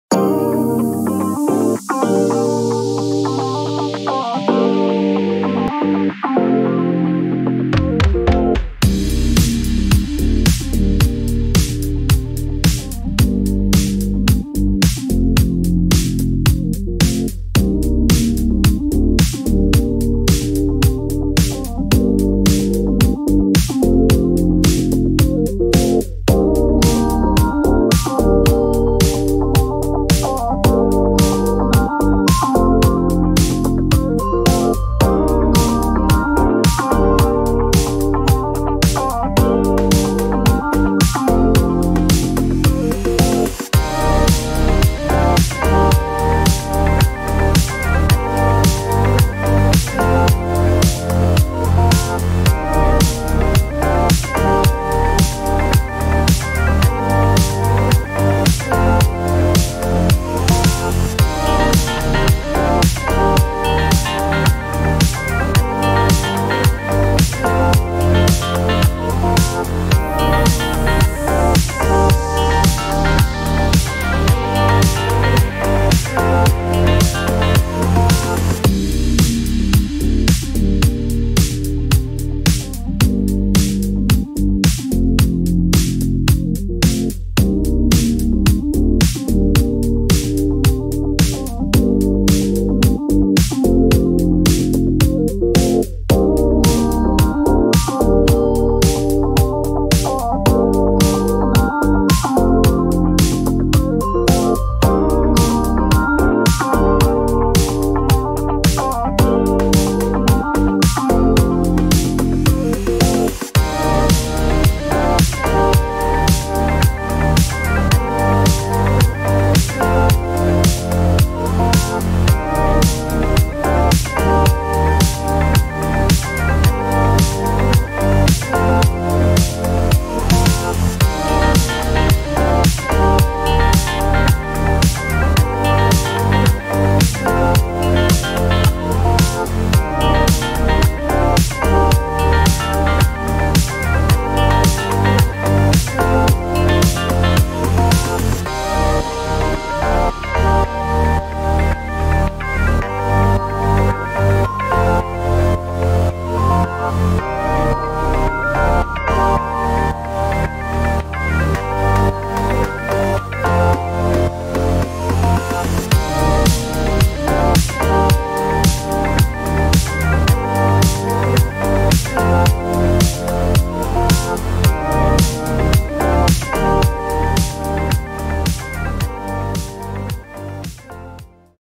Timelapse // Wall replacement on McLouis 2022
Wall replacement at the Mclouis Sovereign
Warranty replacement due to blisters on the wall surface.
Exchange made at the Dicar Motorhome in Geel